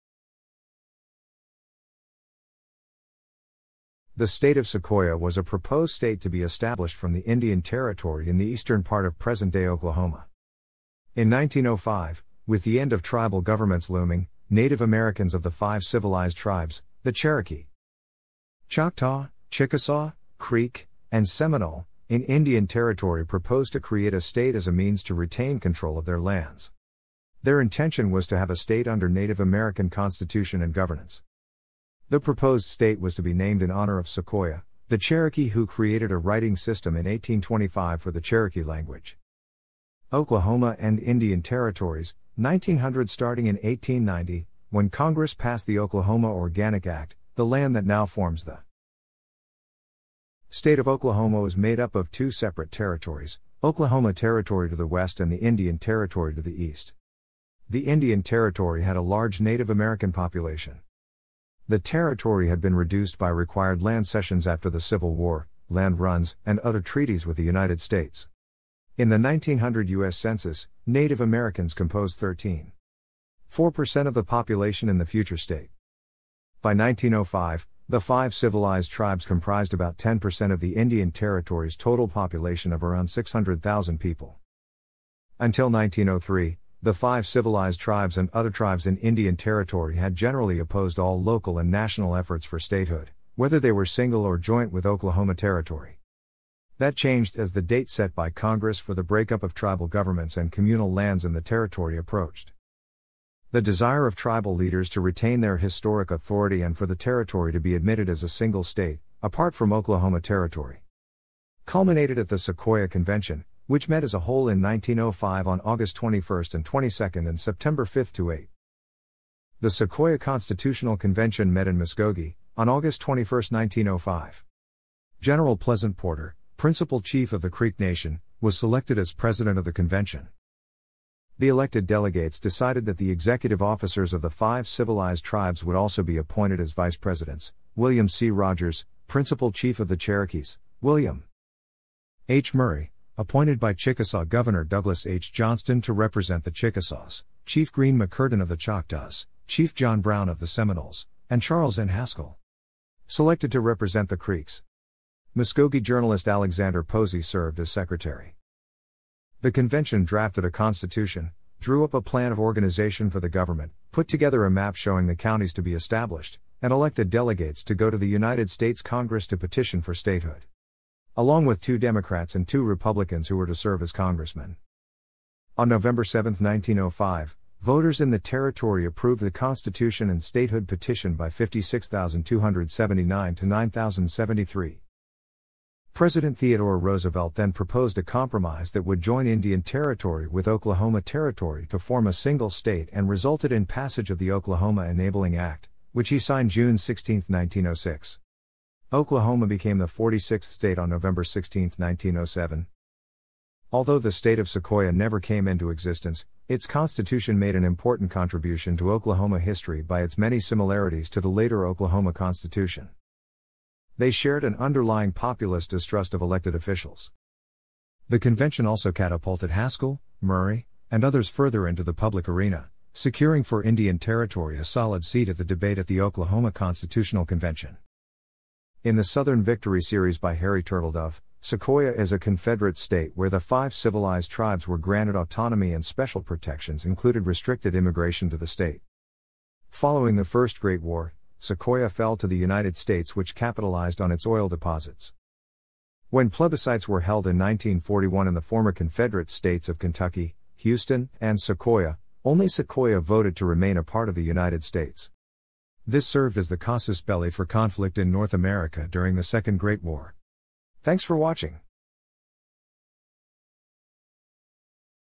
State of Sequoyah
Pre-statehood_history_of_Oklahoma
The State of Sequoyah was a proposed state to be established from the Indian Territory in the eastern part of present-day Oklahoma.
In 1905, with the end of tribal governments looming （as prescribed by the Curtis Act of 1898）, Native Americans of the Five Civilized Tribes—the Cherokee,
Choctaw, Chickasaw, Creek （Muscogee）, and Seminole—in Indian Territory proposed to create a state as a means to retain control of their lands.
Their intention was to have a state under Native American constitution and governance.
The proposed state was to be named in honor of Sequoyah, the Cherokee who created a writing system in 1825 for the Cherokee language.
Oklahoma and Indian Territories, 1900 Starting in 1890, when Congress passed the Oklahoma Organic Act, the land that now forms the
State of Oklahoma was made up of two separate territories: Oklahoma Territory to the west and the Indian Territory to the east.
The Indian Territory had a large Native American population.
The territory had been reduced by required land cessions after the Civil War, land runs, and other treaties with the United States.
In the 1900 US Census, Native Americans composed 13.
4 percent of the population in the future state.
By 1905, the Five Civilized Tribes comprised about 10% of the Indian Territory’s total population of around 600,000 people.
Until 1903, the Five Civilized Tribes and other tribes in Indian Territory had generally opposed all local and national efforts for statehood, whether they were single or joint with Oklahoma Territory.
That changed as the date set by Congress （March 4, 1906） for the breakup of tribal governments and communal lands in the territory approached.
The desire of tribal leaders to retain their historic authority and for...